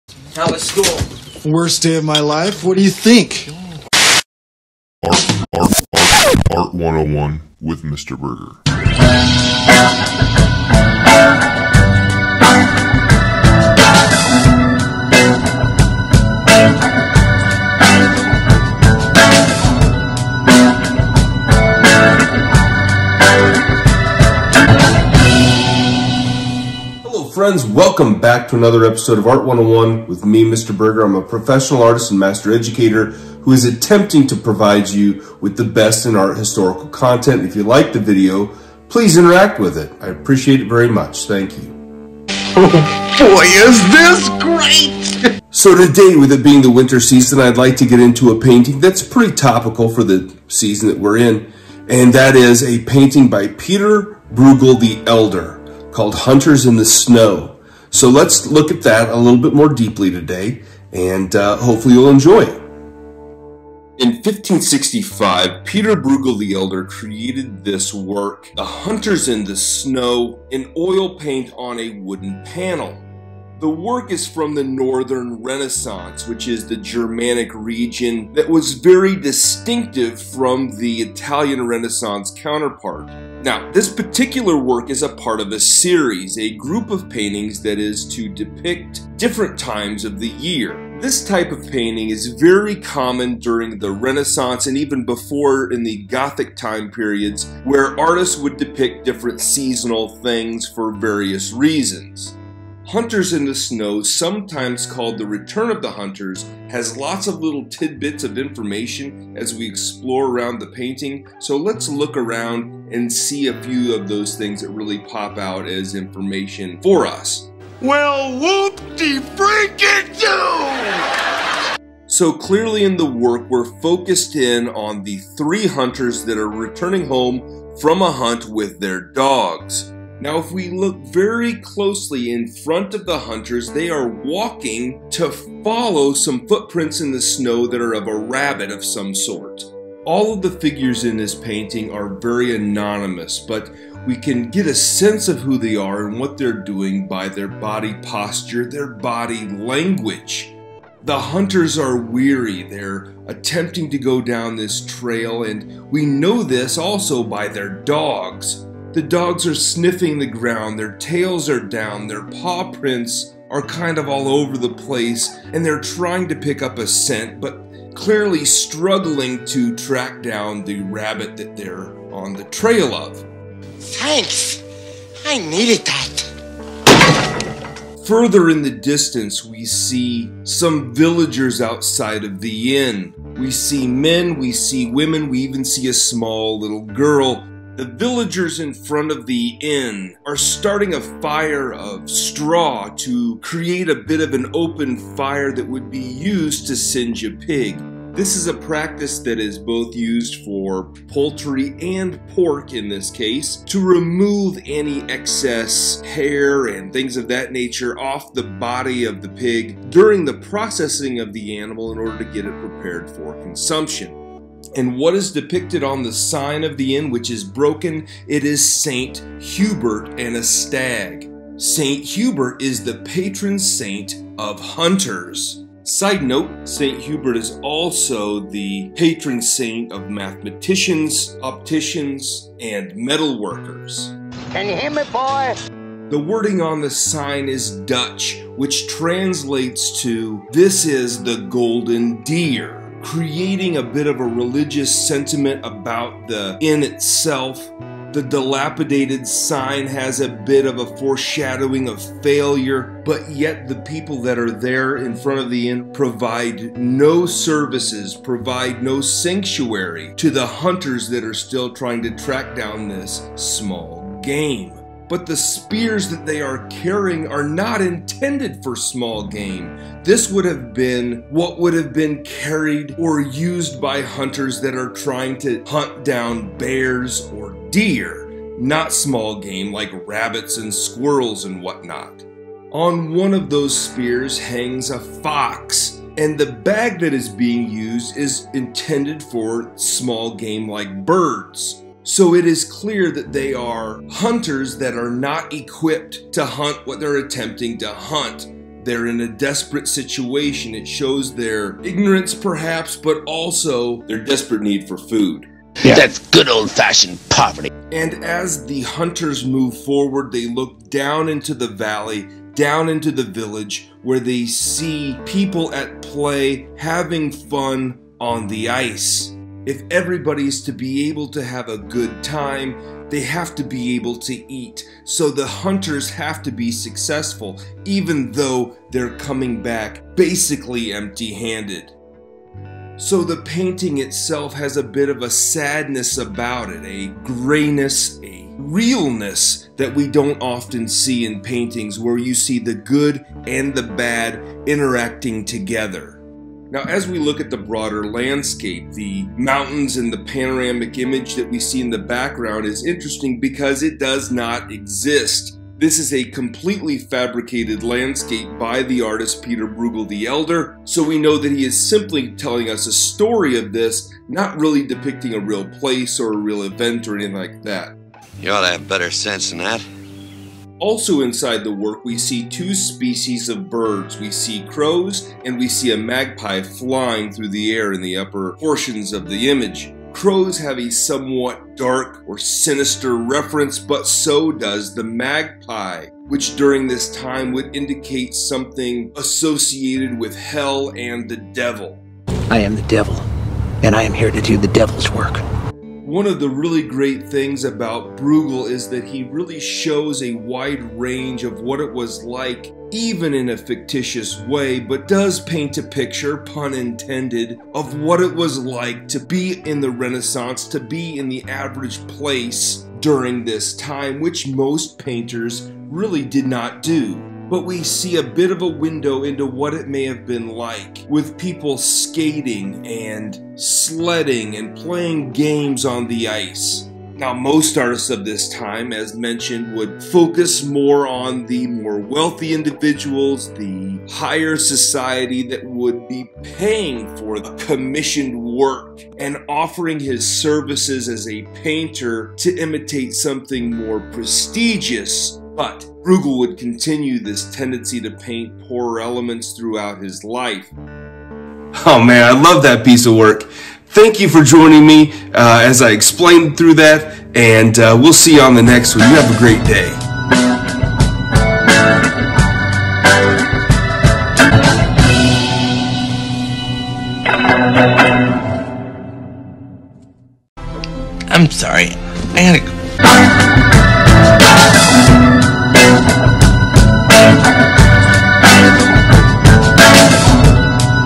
Hunters in the Snow | Art 101
One of the most unique works to come from the Northern Renaissance was the Hunters in the Snow created by Pieter Bruegel the Elder. Check out Mr. Burgher's analysis of this master work.

0:00 - Welcome Scholars
0:45 -  Introduction to Hunters in the Snow
1:03 - Background Knowledge
2:01 - The Hunters
2:57 - Villagers & St. Hubert
3:55 - The Hunt
5:07 - Deeper Message
6:10 - Birds Symbolism
6:35 - Bruegel's Motive
7:40 - End Credits

✉️🐌📨 Snail Mail 📨🐌✉️
Mr. Burgher